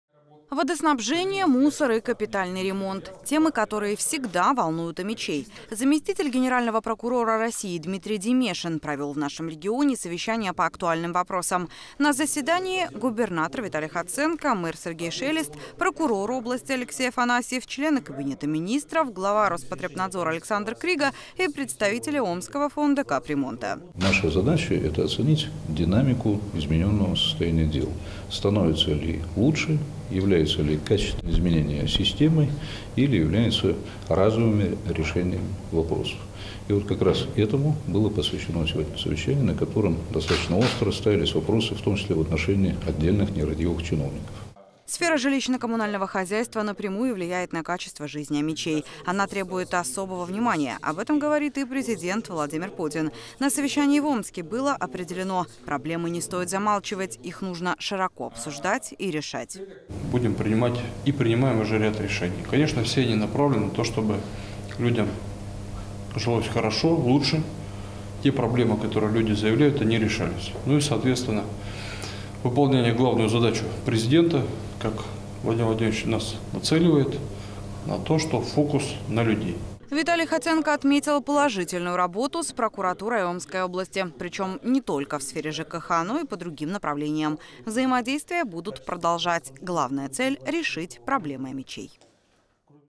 О самом главном
Заместитель Генерального прокурора России Дмитрий Демешин провел в нашем регионе совещание по актуальным вопросам. Какие темы больше всего волнуют омичей?